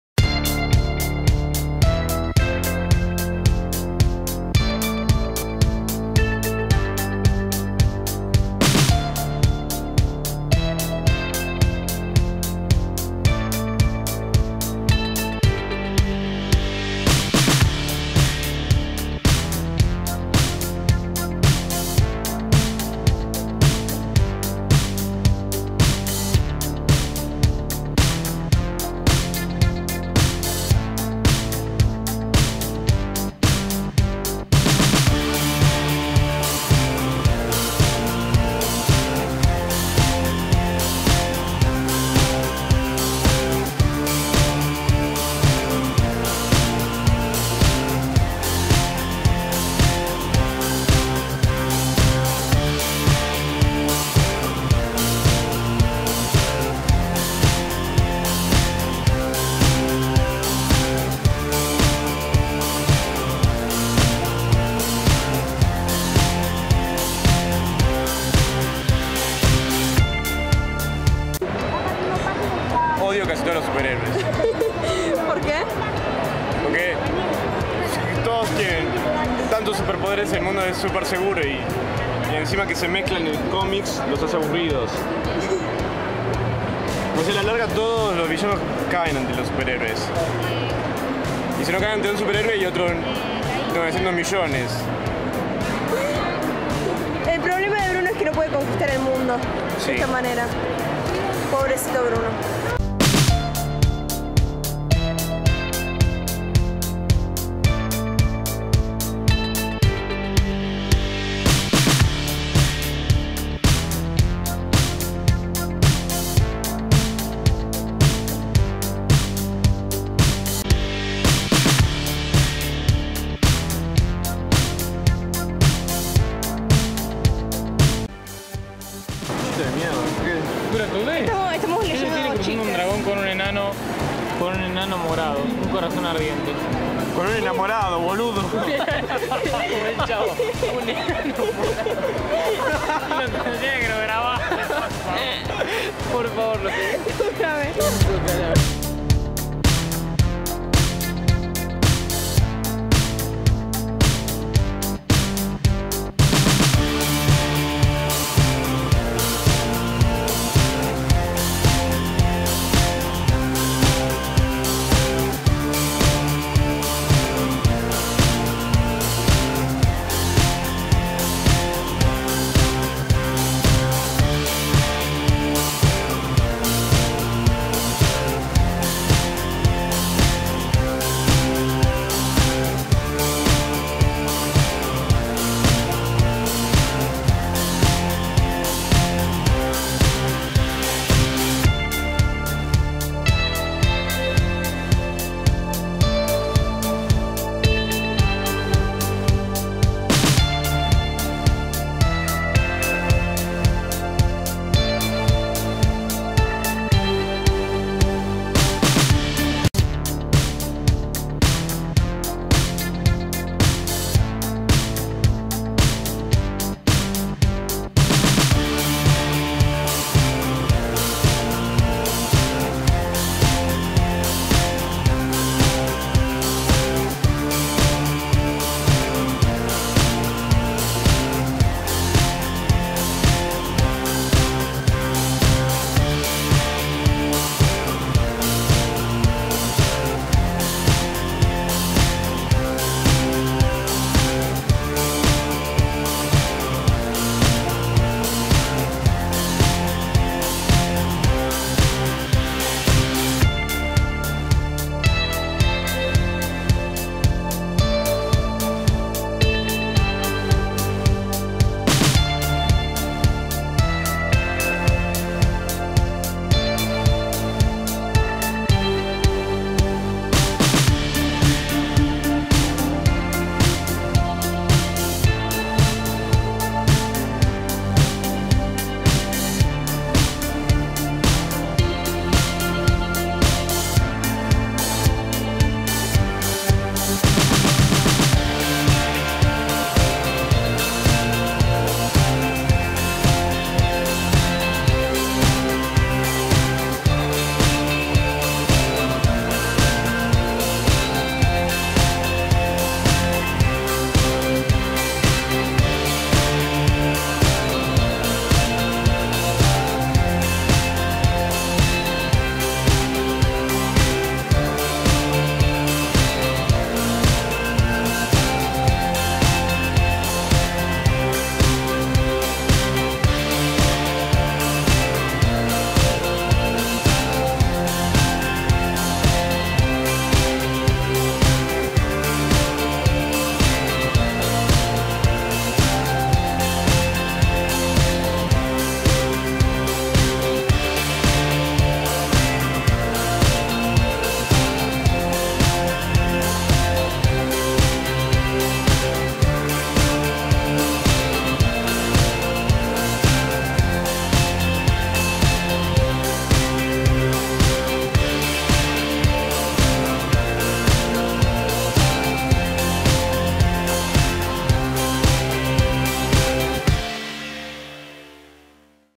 The Book Fair 2014
So i should've edited this like two weeks ago... and uploaded it last week right after editing but.. well, you know me (sorry)

Anyway, every year there's this huge book fair in Buenos Aires, and i went with some friends, filmed a little bit, and bought a poster and a book.
Also, one of my friends is a big fan of Maze Runner, and we tried meeting James Dashner. We queued for the book signing but there was so many people that we kinda gave up; despite this, we were able to see him from afar... oh, well, not everything is posible, and my friend met him the following day at another book signing at a bookstore. So the story has a happy ending.

Wow, i never said so many "books" in one paragraph.
Also, music not mine, got it from the YouTube library (thanks YouTube)